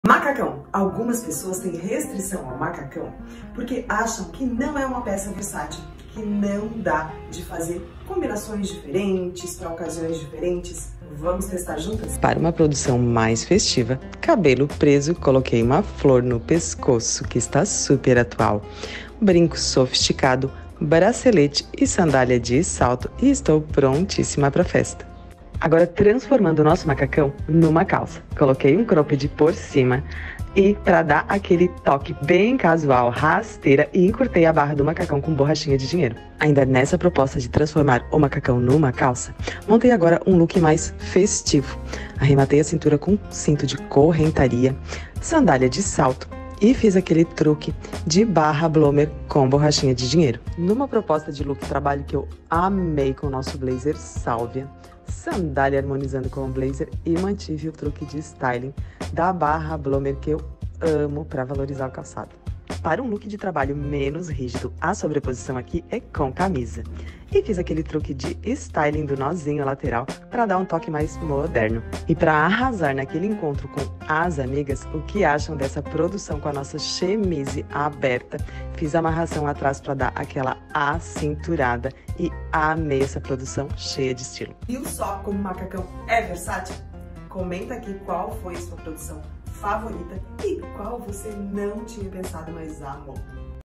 A Versatilidade do Macacão
Você já imaginou que um macacão poderia ser tão versátil? 😱

De uma tarde casual a um evento especial, o macacão é a escolha perfeita. 🌟

Descubra como você pode estilizar essa peça única de várias maneiras. 😍

Qual é o seu styling favorito? Comenta aqui!